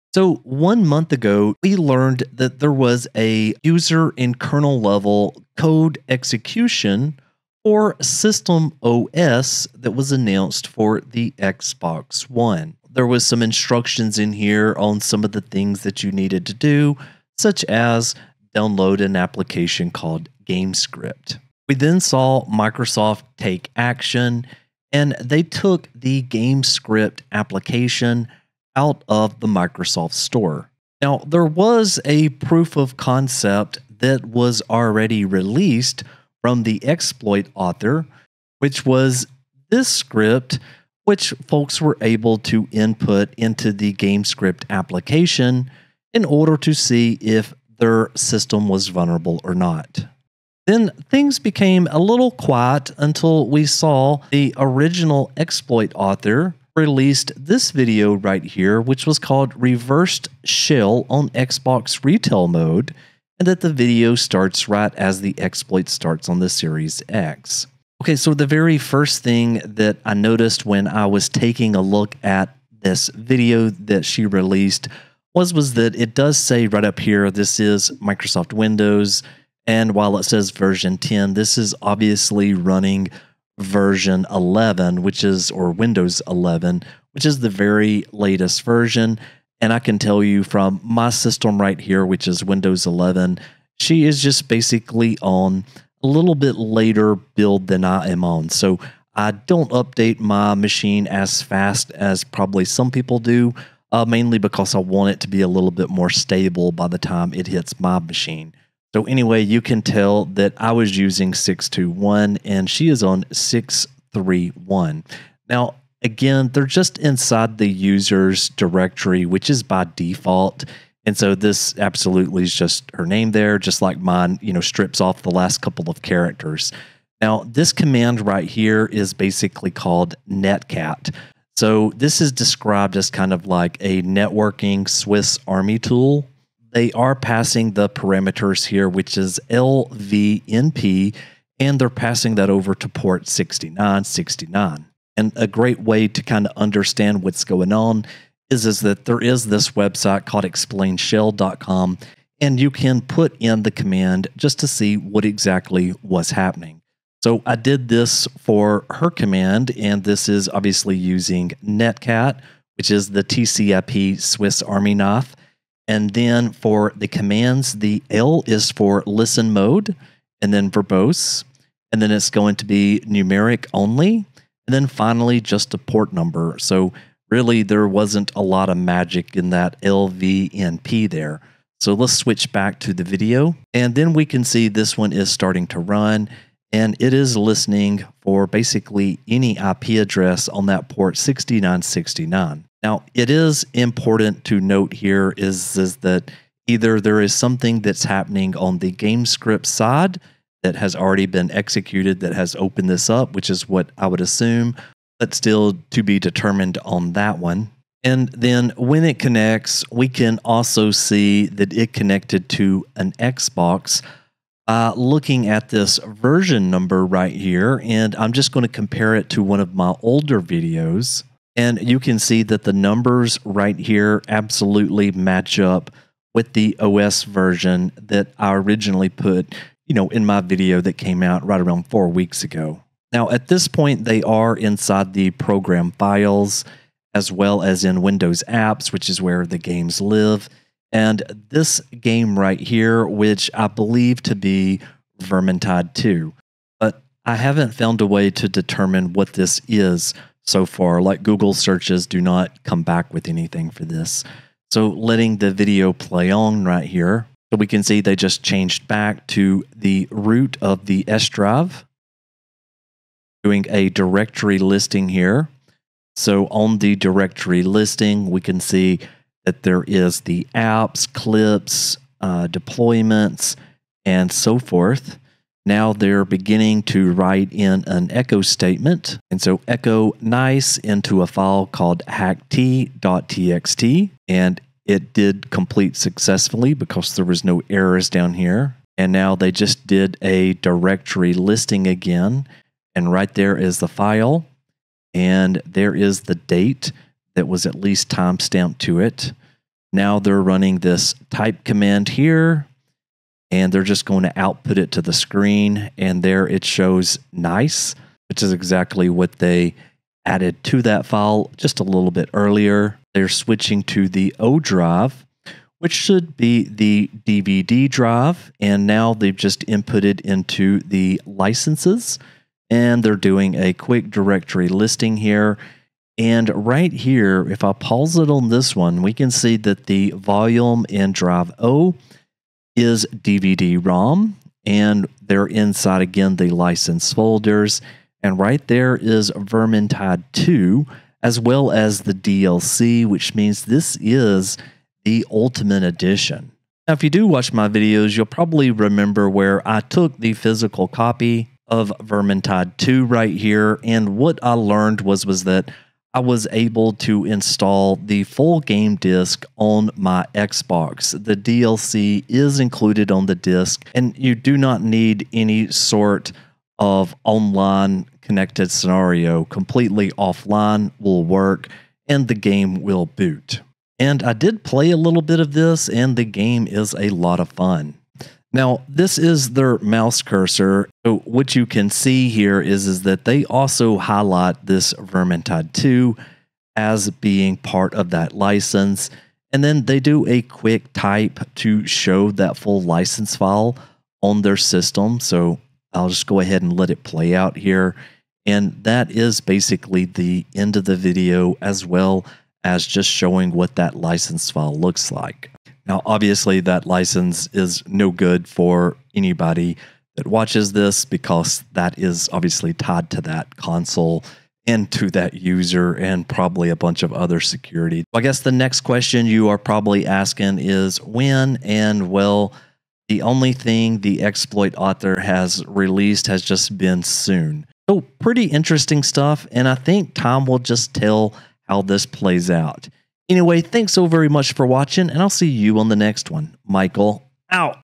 Taking a look at the Reverse Shell on Xbox Retail Mode
News recap | Xbox One Research

(19) emma on X: "reverse shell on xbox retail mode (video starts right as the exploit starts on the series x) / X

(20) mbcrump - PlayStation 4/5 Homebrew Tutorials on X: "If things work out with this Xbox One hack, then this game is going to be very popular for those who want to dump their games. / X

(58) emma on X: "ok i’m gonna be normal now! time to use exploits for the one truly noble cause: hacking game consoles 🥲" / X

(26) emma on X: "kaslr no longer an issue!!! took longer than i had hoped but fine tuned the side channel to get the kernel base and then corrupting some convenient globals and now we’re back to being able to leak object addresses! progress~~~ / X